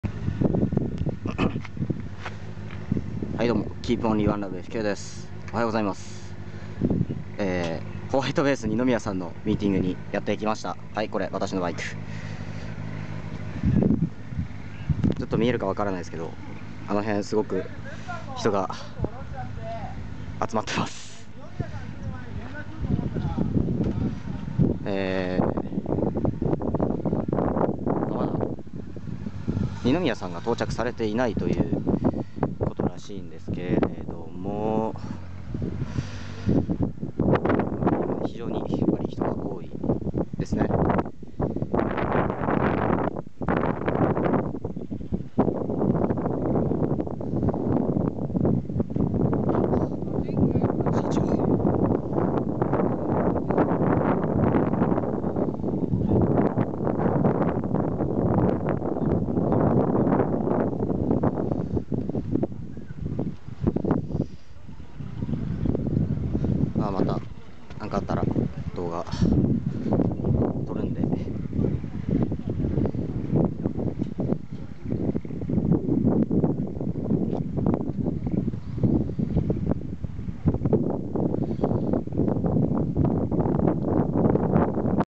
ホワイトベースのミーティングに潜入してました。
乃木坂46の握手会の前にこっそり潜入してきました。
声が聞き取りづらかったらすみません。